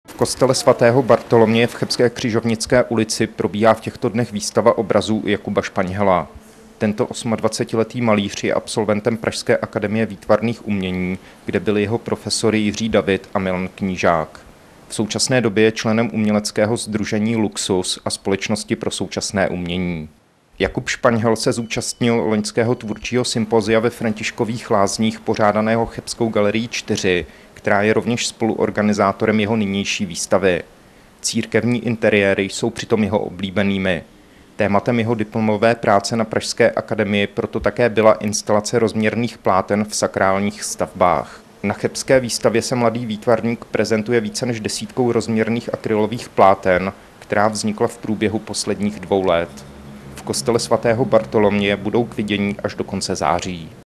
2004 – Cheb: V Bartoloměji vystavuje Jakub Špaňhel (TV Západ)
V kostele sv. Bartoloměje v chebské Křižovnické ulici probíhá v těchto dnech výstava obrazů Jakuba Špaňhela. Tento osmadvacetiletý malíř je absolventem pražské Akademie výtvarných umění, kde byl jeho profesory Jiří David a Milan Knížák.

V současné době je členem uměleckého sdružení Luxus a Společnosti pro současné umění. Jakub Špaňhel se zúčastnil loňského tvůrčího sympozia ve Františkových Lázních pořádaného chebské Galerii 4, která je rovněž spoluorganizátor jeho nynější výstavy. Církevní interiéry jsou přitom jeho oblíbenými. Tématem jeho diplomové práce na pražské akademii proto také byla instalace rozměrných pláten v sakrálních stavbách.